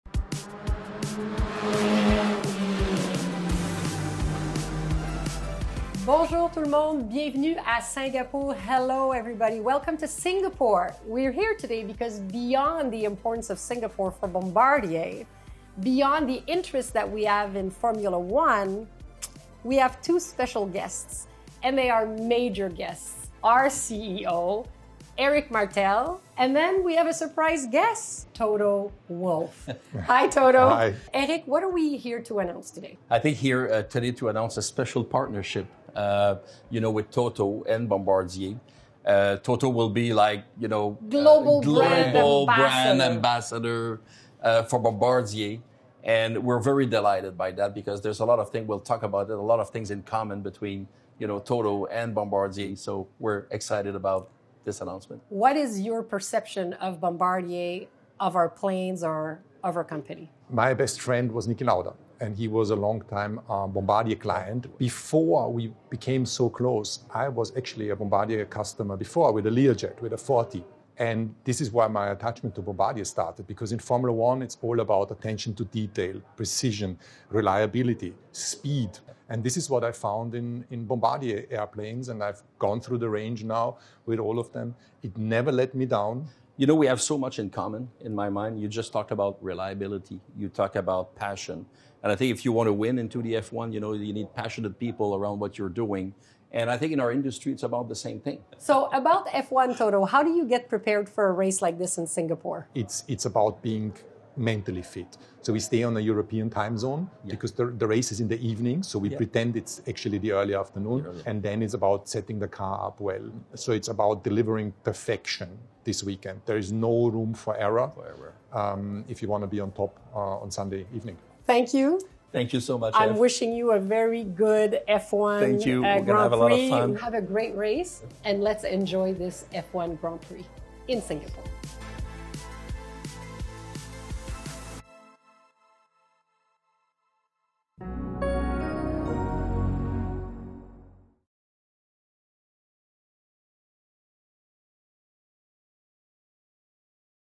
Présentation de Toto Wolff comme ambassadeur mondial de marque
Notre nouvel ambassadeur mondial de la marque, Toto Wolff, est un leader visionnaire et un entrepreneur qui incarne l'esprit d'innovation, d'excellence, de performance et de compétitivité. Regardez cette interview dans laquelle il explique pourquoi il choisit les performances des avions Bombardier depuis de nombreuses années.

Bonne chance ce week-end au SingaporeGP !